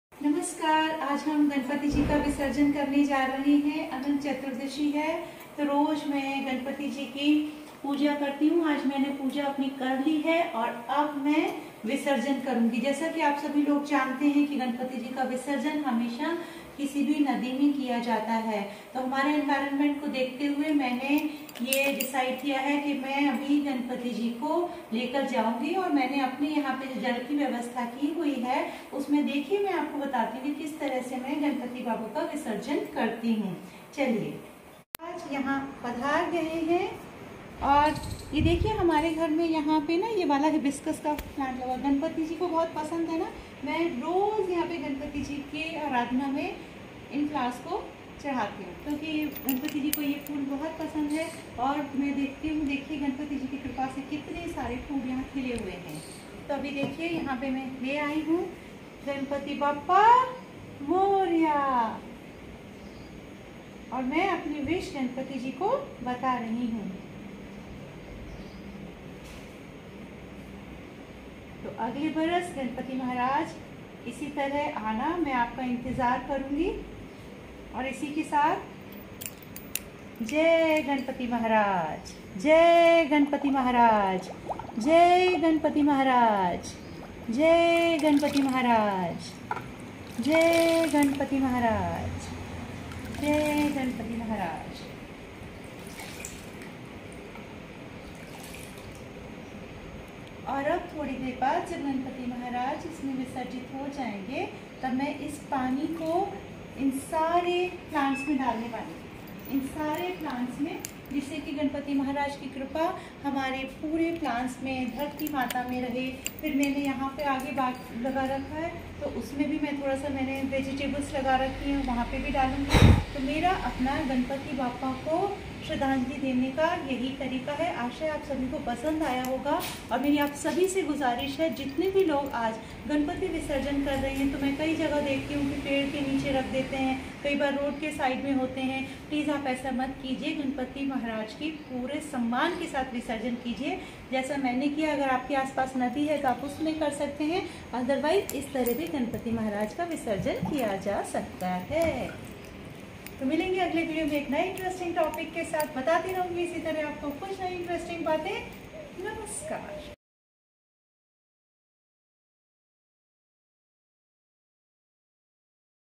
गणपति जी का विसर्जन सही तरीका मन की इच्छा पूरी होगी Dr.Leena Chauhan
गणपति जी का विसर्जन इस तरह करें। सारी इच्छाएं पूरी होती हैं।मन शांत रहेगा। तनाव कभी नहीं होगा। मां प्रकृति खुश रहें इससे अच्छा तो कुछ भी नहीं है।
भगवान जी हम सब पर कृपा बनाए रखे। अगले साल गणपति जी फिर आना।
Dr.Leena Chauhan
Family Counsellor
Bubbly Family Counselling center
Noida